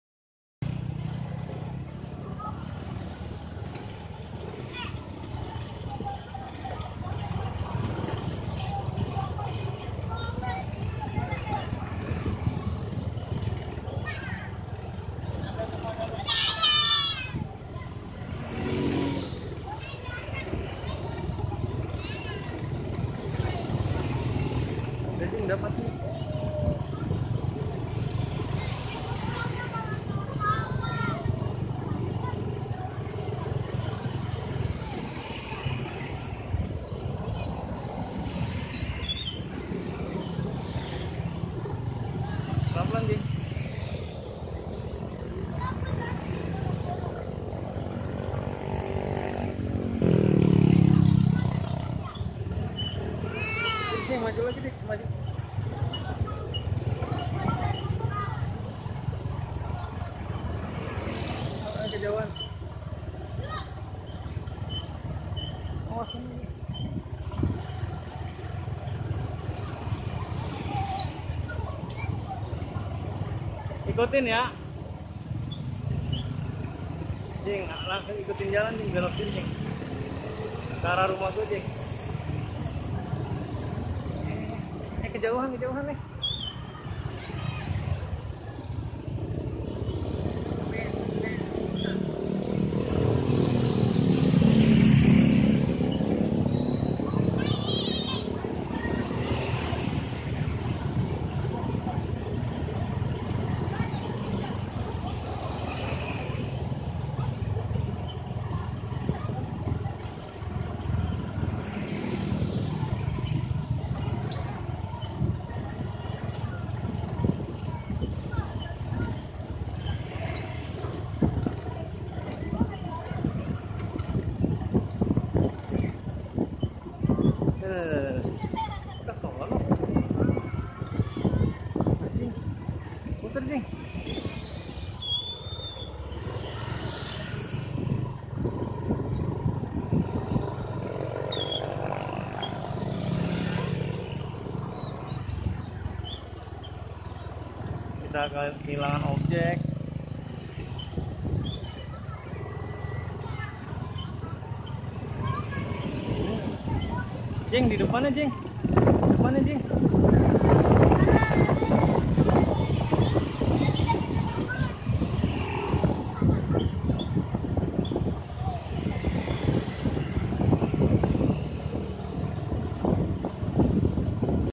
Pengetesan kereta gajah Pare-Pare | odong-odong kereta motor gajah
Berikut ini adalah video pengetesan kereta gajah untuk dikirimkan ke Pare-Pare, Sulawesi Selatan.
Info pemesanan silakan hubungi Bang Hendra (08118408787)